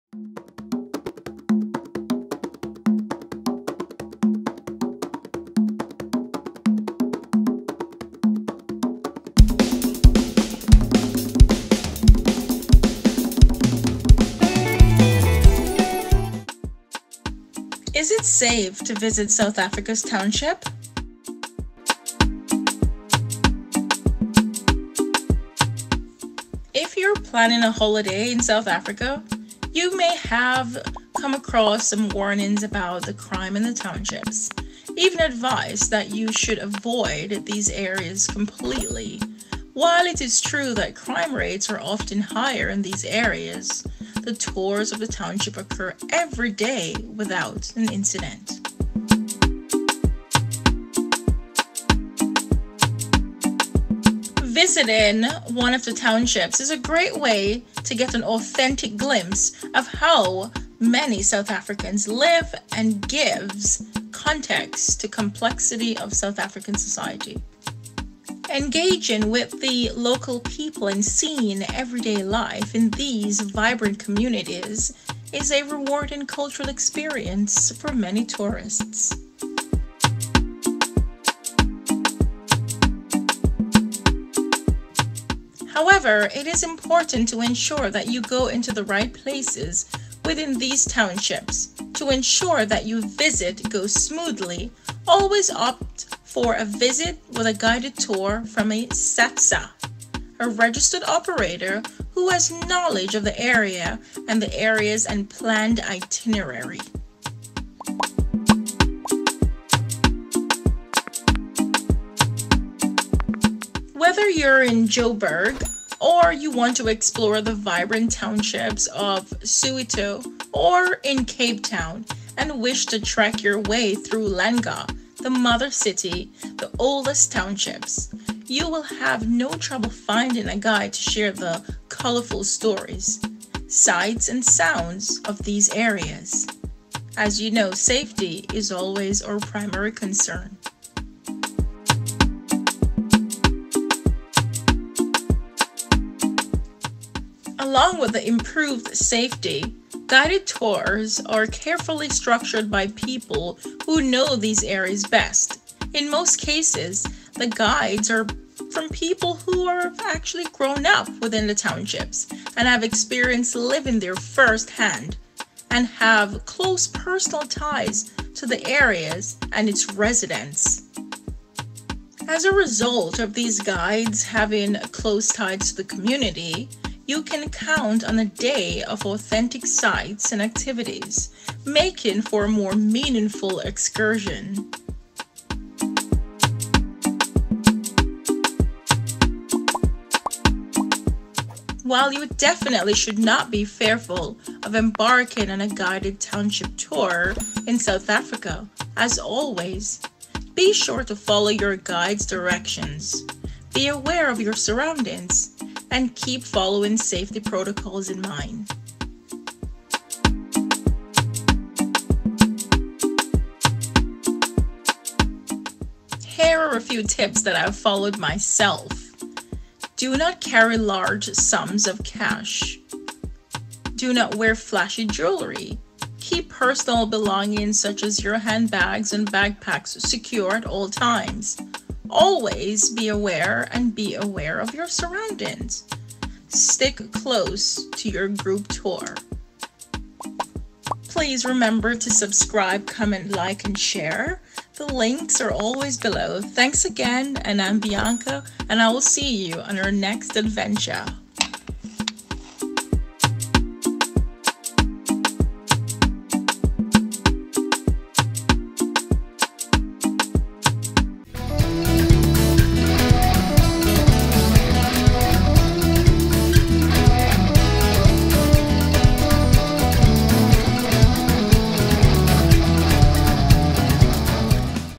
Township Tales: Navigating South Africa's Culture with Safety Tips
🌍 Embark on a cultural odyssey as we delve into South Africa's townships, unraveling the rich tapestry of traditions, and offering essential safety tips. Join us in this immersive experience, where we bridge gaps, share insights, and celebrate the vibrant spirit of the townships. 🏠👫